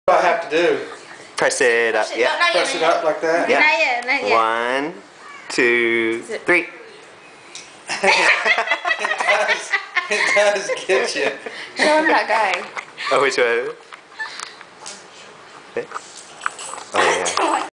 a shocking experience 10: the mister
he knew what was coming: not a big reaction but you could see him jump from the jolt